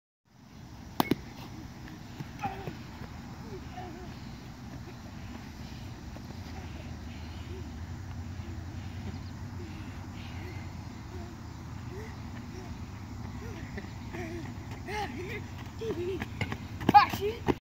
The run down
Tried to stretch a triple into a HR